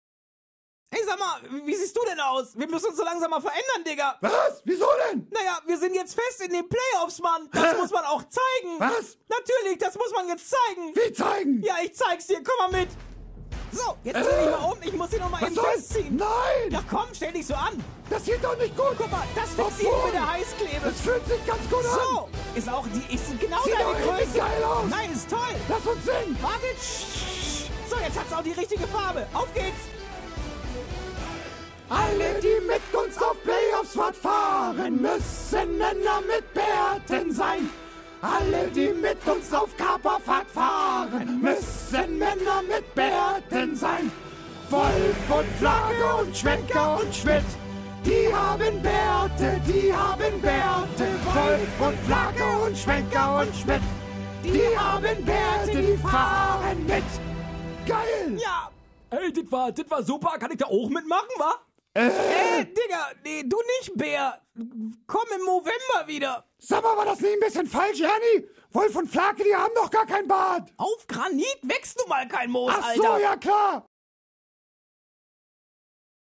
Freezersstreet Playoffs 2014 - Wolf und Flaake und Schwenker & Schmidt
Die Hamburg Freezers haben die Playoffs 2014 gesichert - Grund genug für ein paar Veränderungen in der Freezersstreet.